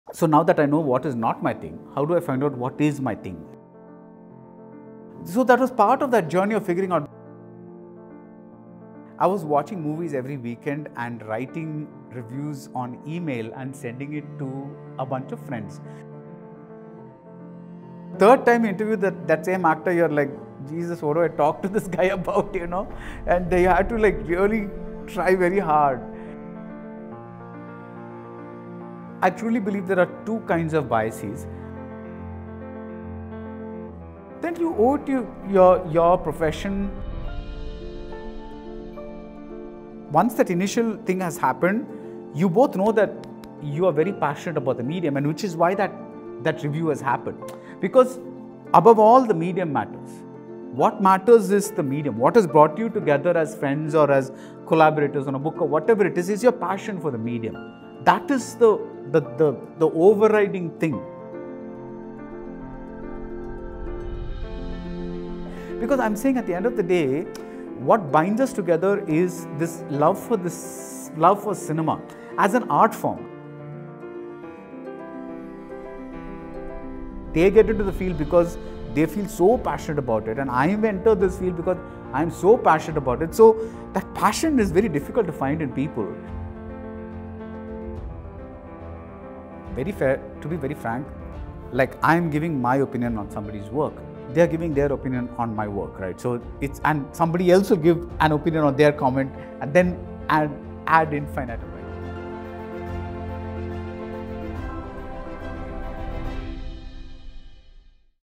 Snippets from Exploring the Mindscape with Baradwaj Rangan.
He spoke about how he found his passion, his love for cinema, portrayal of mental health and more.
The first part of the interview will be published tomorrow.